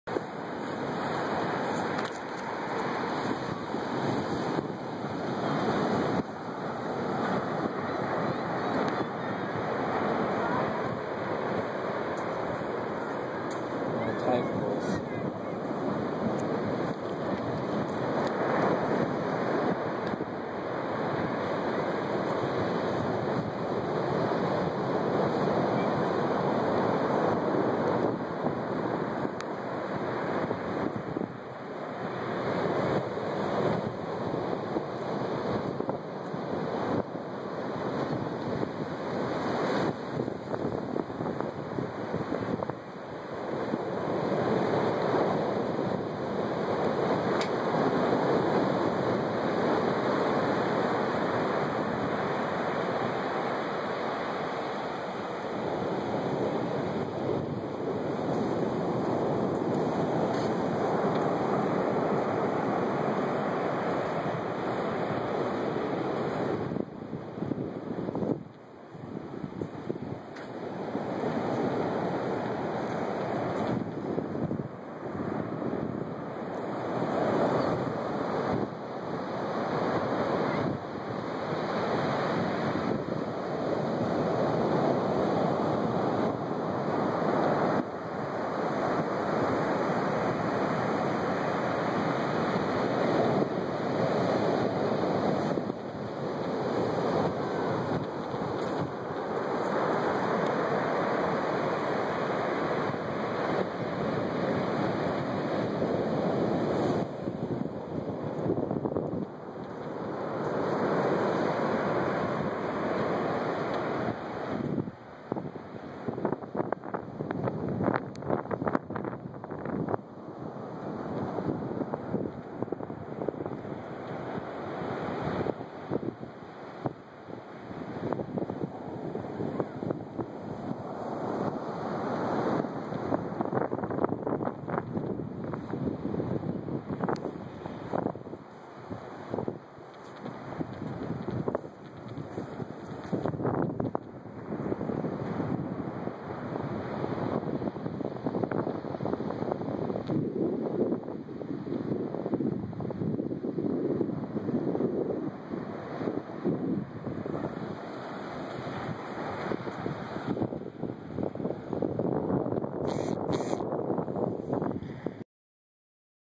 The Hampton Beach Black Friday 2013
The Hampton Beach
on Black Friday 2013
For civic engagement by cleaning up the beach with the family and enjoying the grand site instead of shopping and forcing people to slave for material economics that destroy our oceans and beautiful beaches...
As a seagull eats a washed up clam ^^